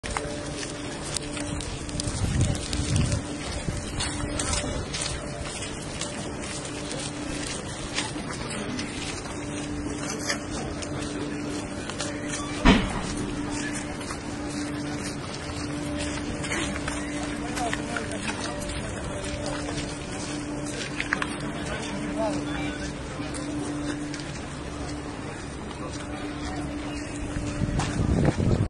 Russian Ukraine War 26/02/2022. A residential building in Kyiv after a Russian missile attack
Ukraine, Russia, War, Ucrania, Rusia, Guerra, Krieg, Oekraïne, Rusland, Oorlog, Krig